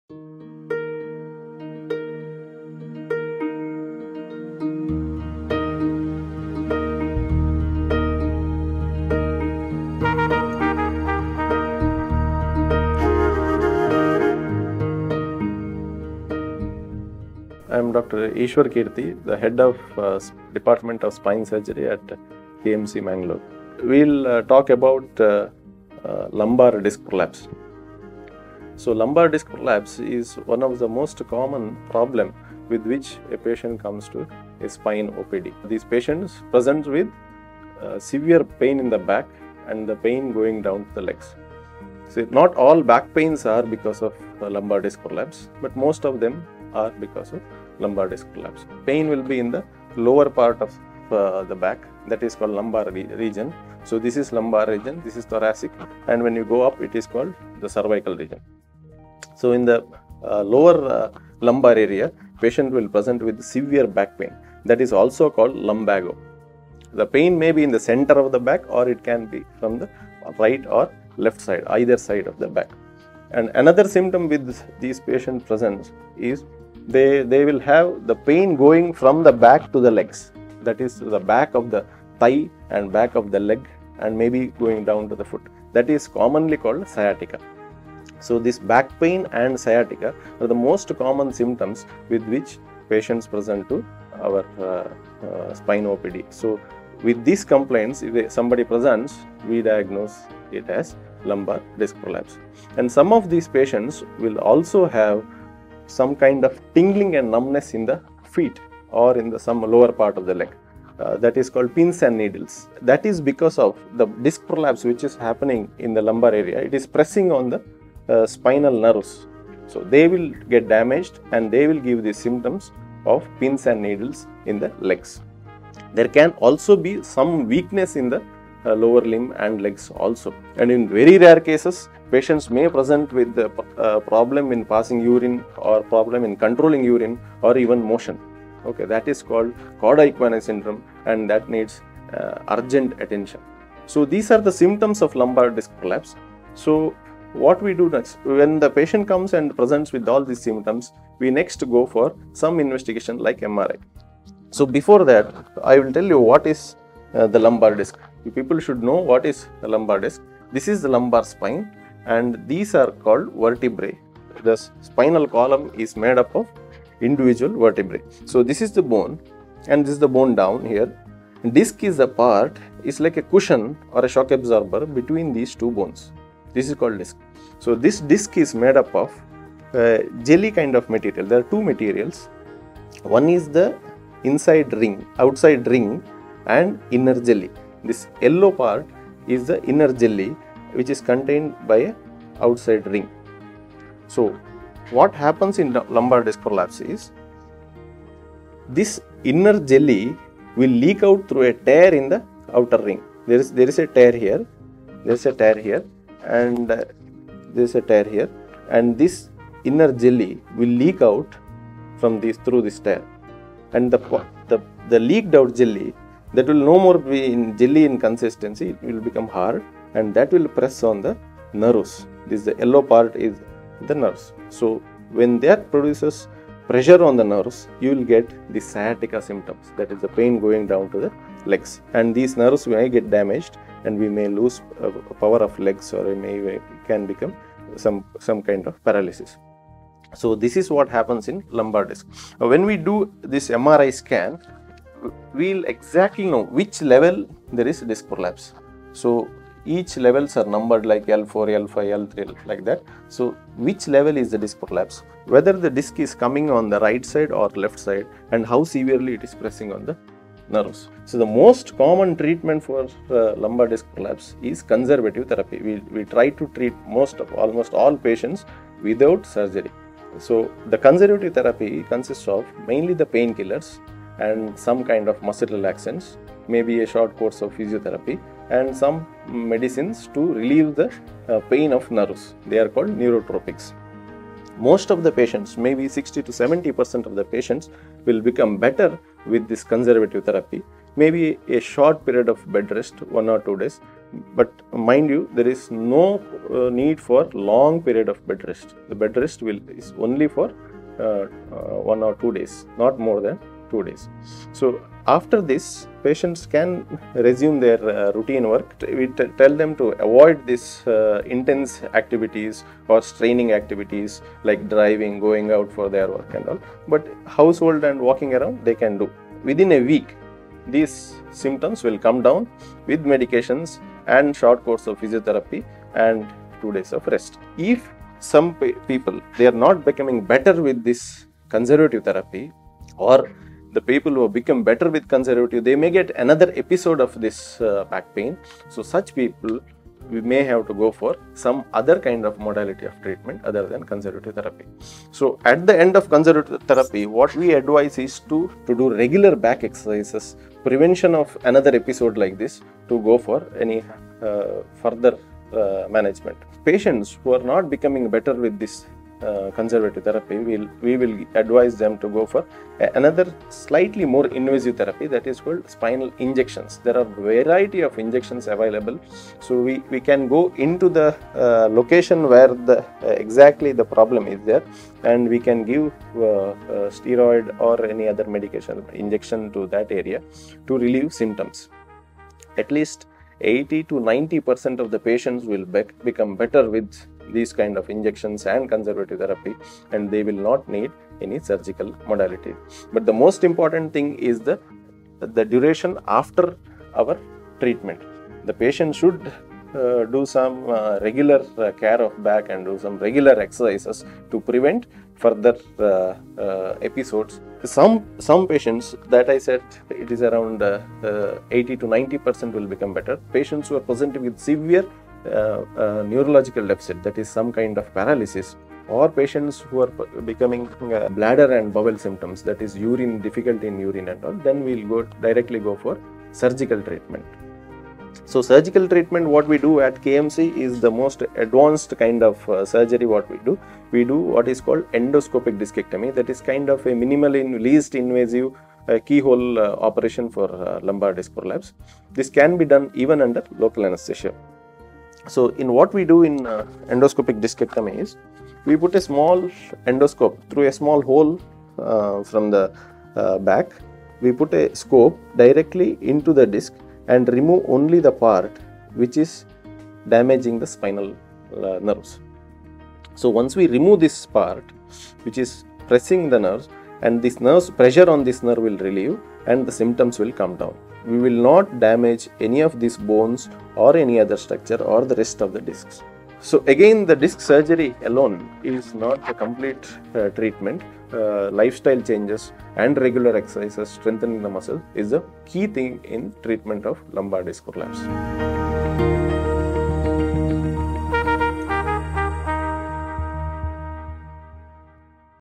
Dr. Ishwar Keerthi on Disc Prolapse | L4 L5 disc Prolapse Symptoms & Treatment | Manipal
Chronic pain from the lower back to the back of the legs might be a symptom of lumbar disc prolapse, says Dr. Ishwar Keerthi, Consultant- Spine Surgeon, KMC Hospital, Mangaluru explains what is lumbar disc prolapse, l4 l5 herniated disc symptoms & lumbar disc prolapse causes and how what is l4 l5 disc prolapse treatment can be done. Watch here to know more!

l4 l5 disc prolapse treatment, l4 l5 herniated disc symptoms, lumbar disc prolapse causes, what is lumbar disc prolapse